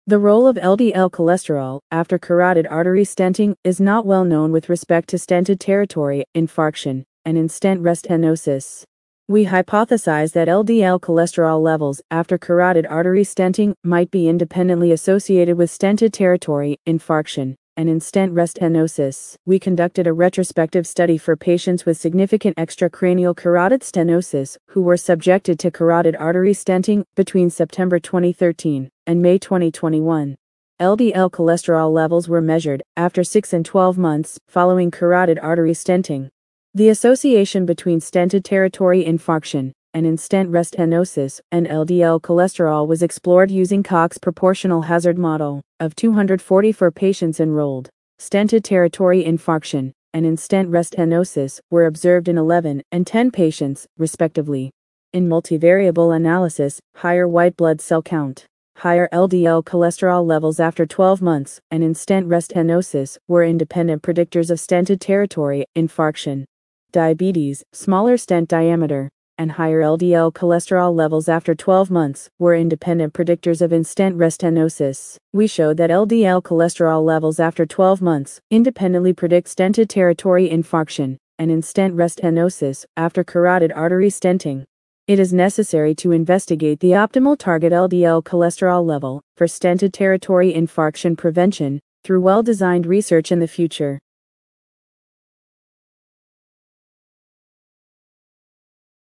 LDL-C Levels Are Associated With Subsequent Stented-Territory Ischemic Stroke After CAS
J Lipid Atheroscler. 2023 May;12(2):175-188. English.

Published online May 08, 2023.